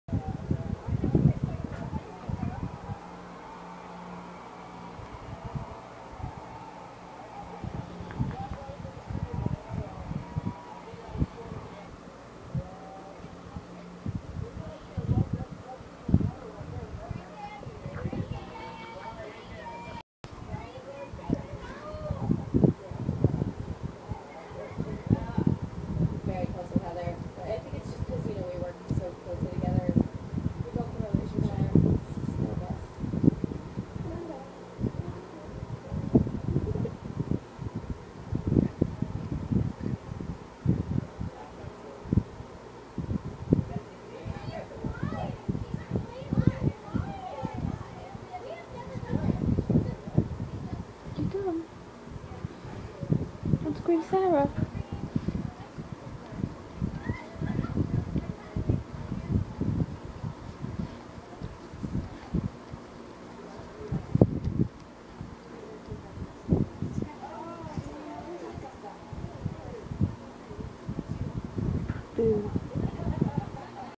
Siberian Husky grooming herself
Lila nibbling herself on my bed this morning. She's nearly 14 months old!